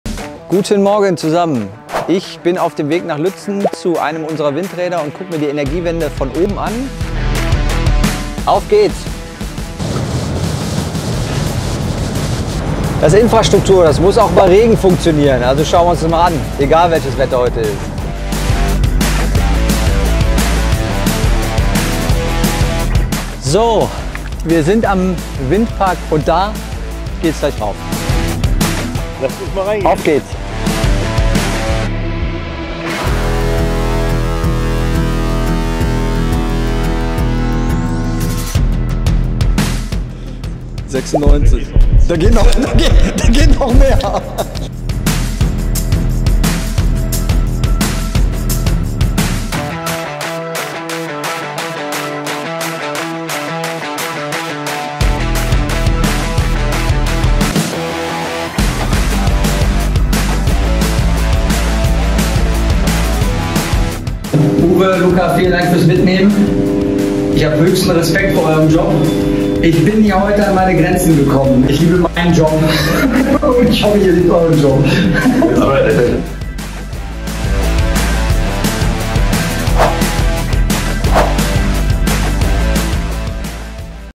In 150 Meter Höhe auf einem Windrad die Energiewende hautnah erleben
„Manchmal muss man seinen Horizont erweitern, um die Welt aus einer neuen Perspektive zu sehen.“ – mit besseren Worten könnte man diese Aktion nicht beschreiben! 🤩 enviaM-CEO Stephan Lowis hat sich in ein echtes Abenteuer im Windpark Lützen gewagt. 👀

Der Blick aus 150 Meter Höhe auf erneuerbare Energiequellen bringt uns nicht nur einen atemberaubenden Perspektivwechsel der anderen Art, sondern auch jede Menge neuer Ideen & Energie für zukünftige Projekte. Der Ausflug hat gezeigt, dass eine nachhaltige Zukunft nicht nur in der Theorie, sondern auch hautnah erlebbar ist. 🎥